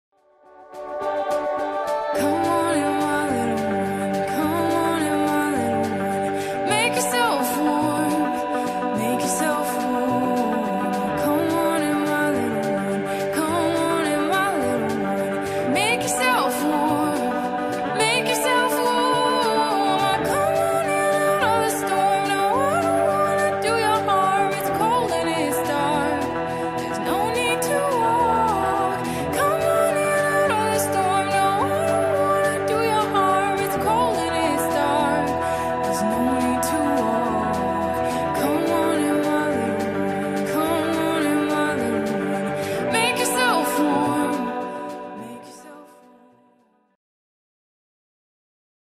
Amazing Great danes.
The world's biggest dog rase tuning & plaging in the Wood.